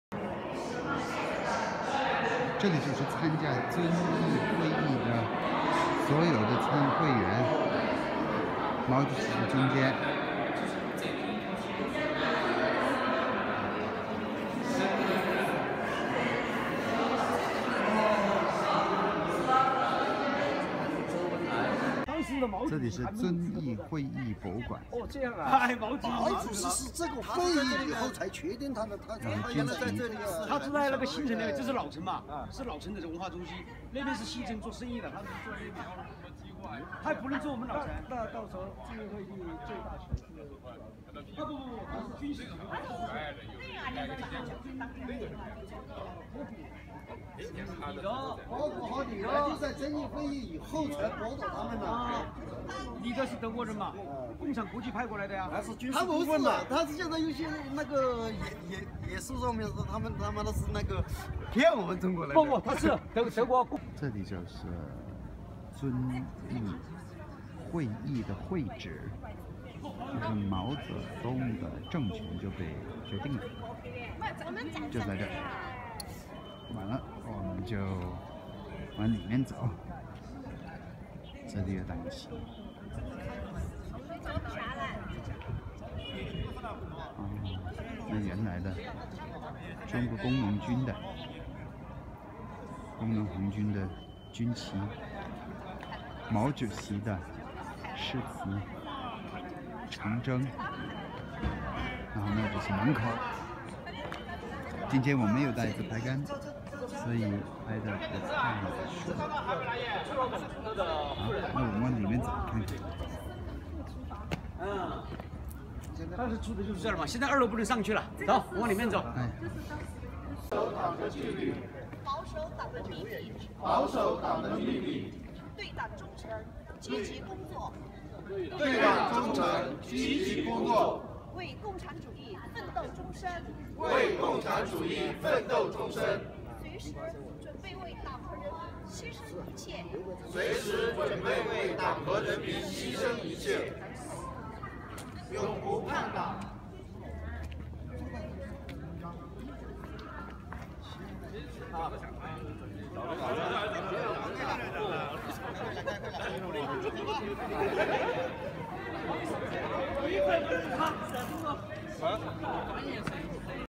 Site of the famous Zunyi Conference 遵义会议会址 -- 贵州省遵义市
The conference was held during the Long March in January 1935 in the second largest city in southern China's Guizhou Province - Zunyi. This is where Mao Zedong was made de facto head of the Party.